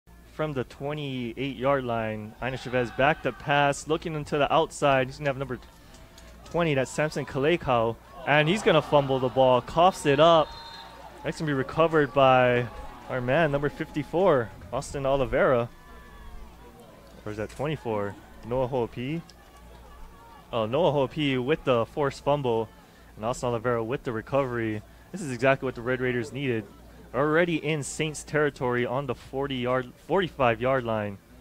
Kauai vs. Saint Francis: Noah Hoopii, forced fumble recovered by A. Olivera (Aug. 12, 2017)
PREVIEW: Kauai visits Saint Francis in non-league clash Saturday —

Video by Tyler Kanoa, Skylar Yamamoto.